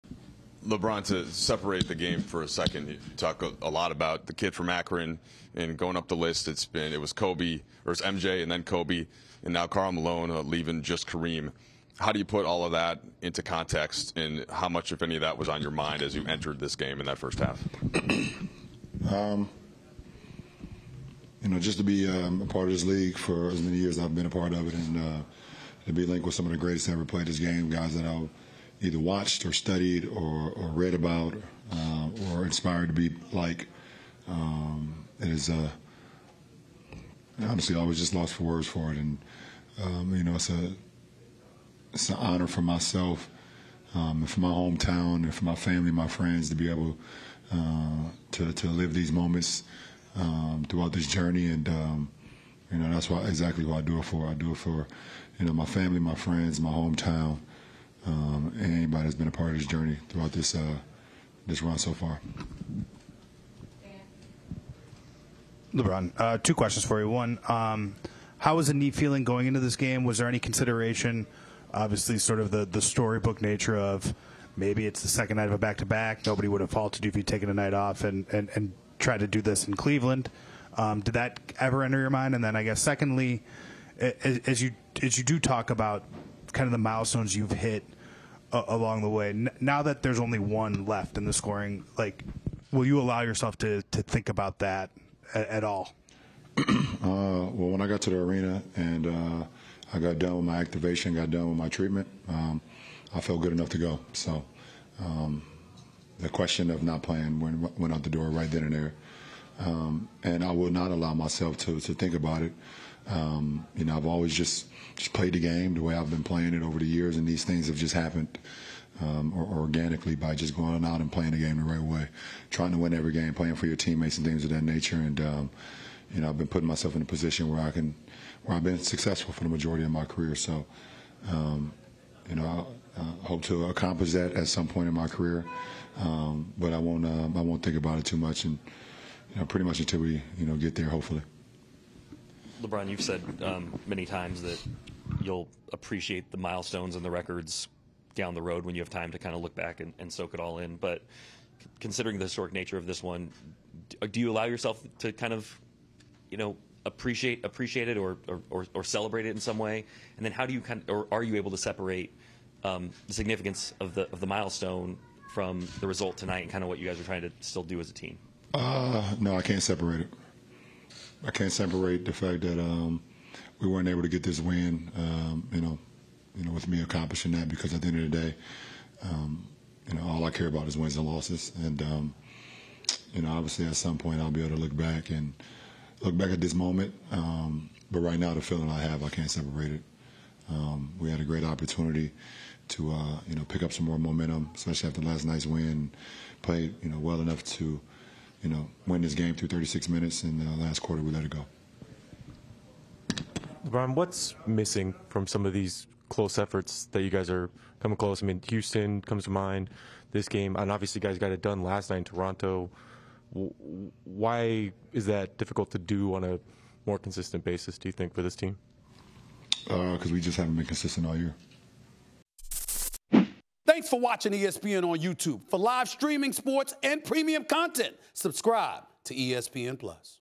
LeBron James on passing Karl Malone, Lakers losing another close game | NBA on ESPN
LeBron James speaks with reporters after the Los Angeles Lakers’ loss to the Washington Wizards. LeBron, who passed Karl Malone for second on the NBA’s all-time scoring list during the game, discusses what it means to him.